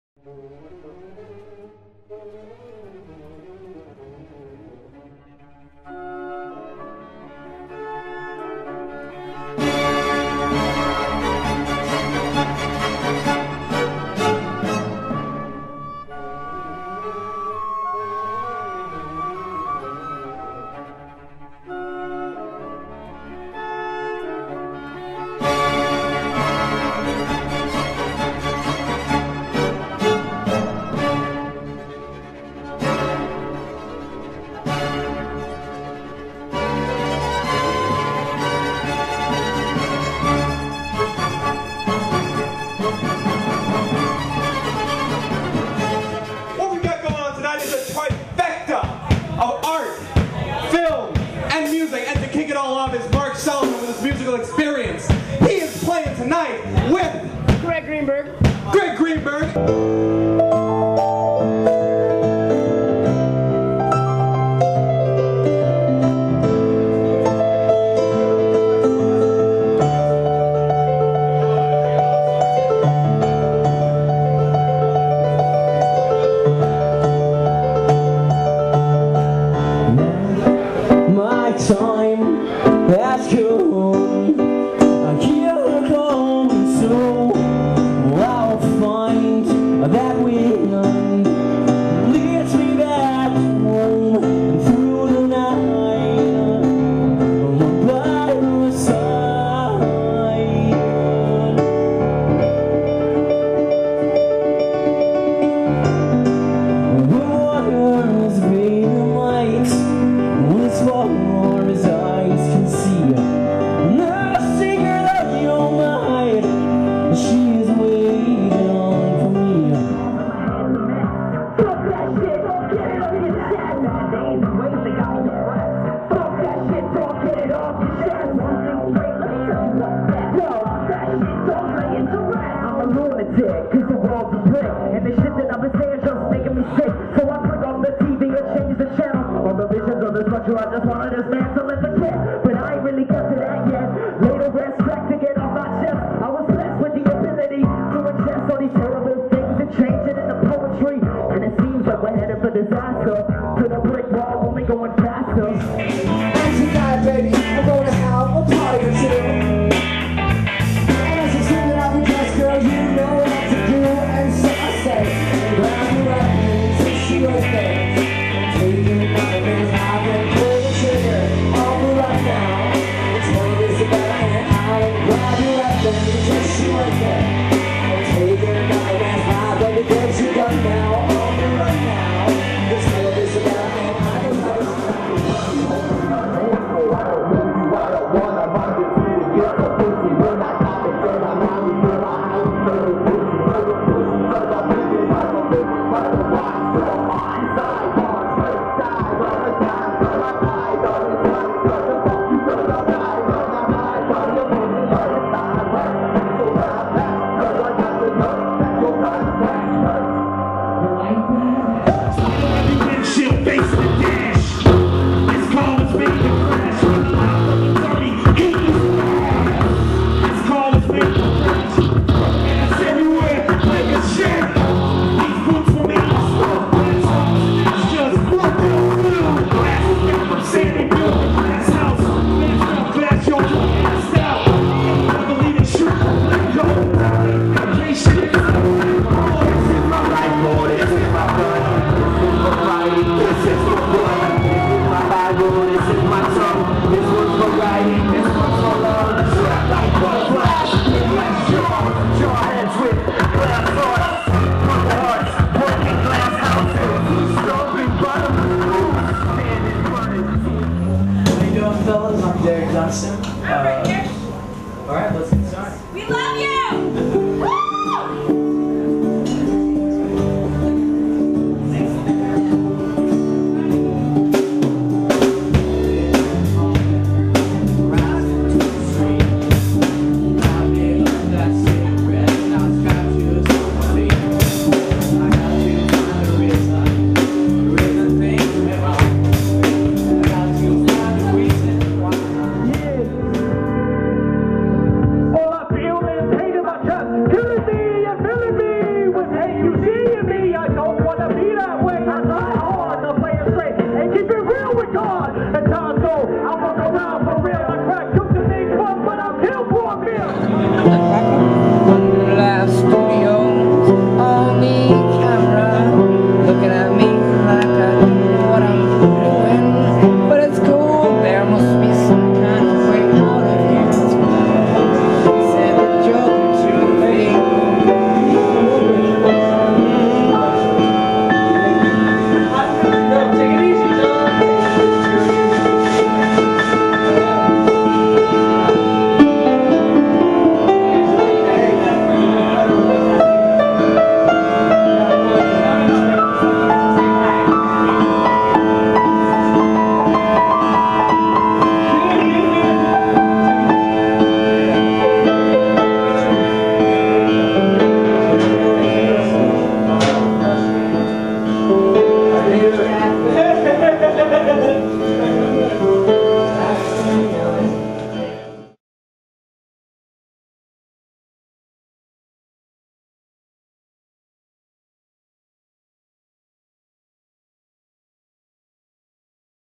Valentines Eve Massacre Florida Horror Short Films Music Art
Valentines Eve Massacre. An Evening of Horror Shorts, Horrorcore Rap, Dark Art Exhibit.